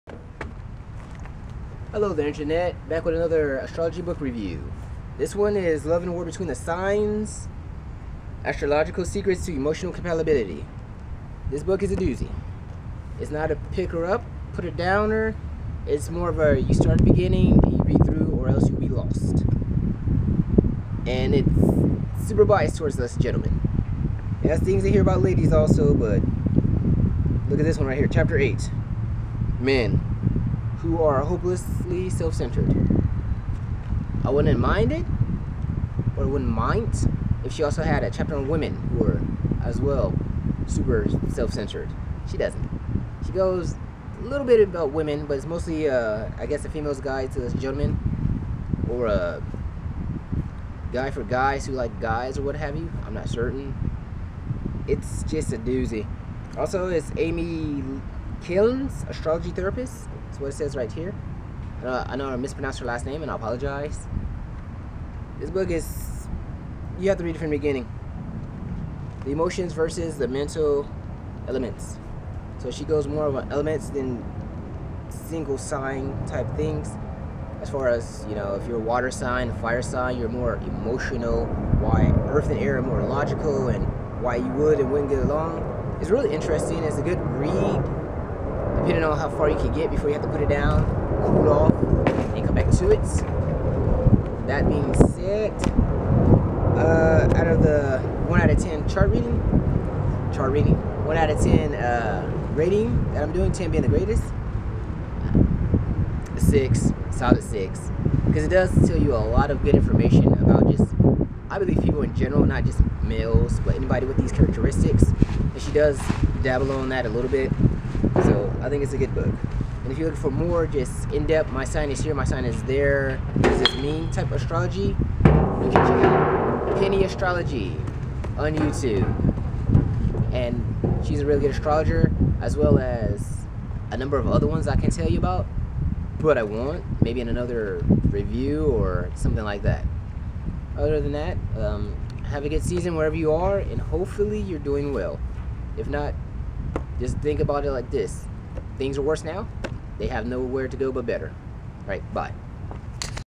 Astrology Book review 2
This book review is different from the last as it's not one you can just jump around with...
Book is "Love & War Between the signs" by: Amy Keehn (Astrological Therapist)